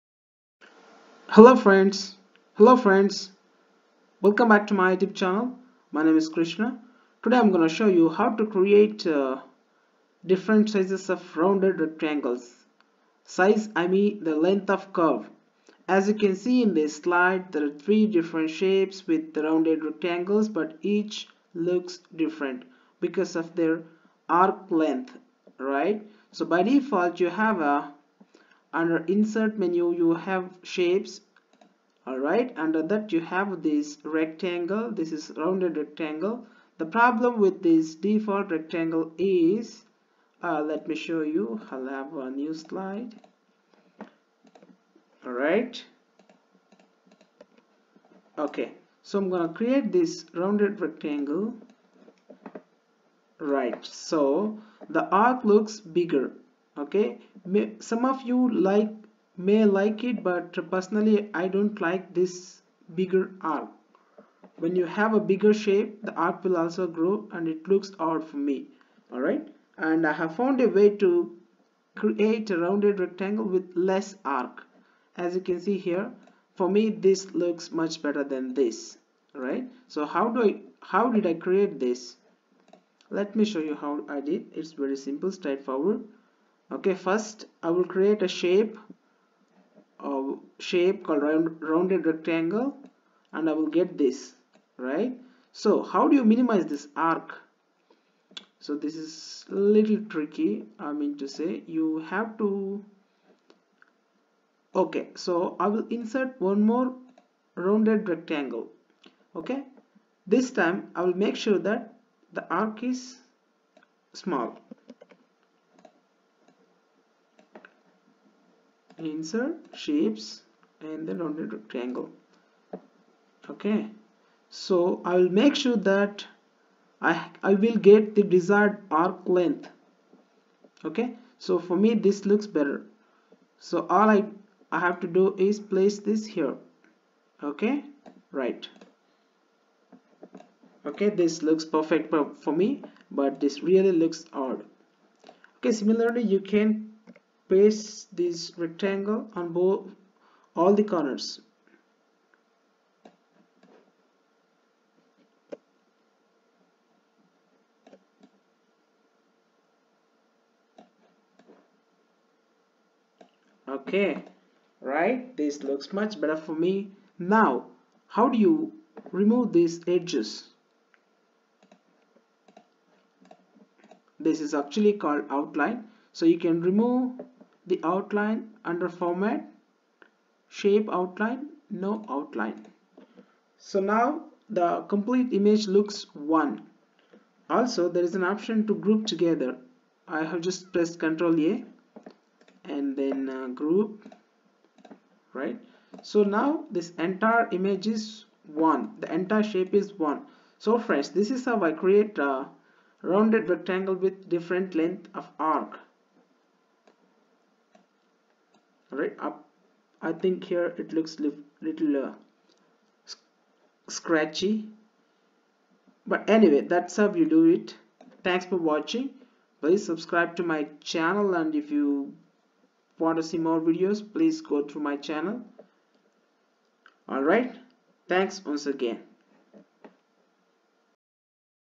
How to create rounded rectangles in ppt
Creating a custom rounded rectangle in power point ppt.

The default rounded rectangle might look odd if the shape you create is too big. The bigger the shape bigger the arc. To minimize the arc length create a smaller rectangle and place them at the corners so that the arc length will decrease.

Then you can remove the shape outline and group all the shapes into one so that it looks like one shape.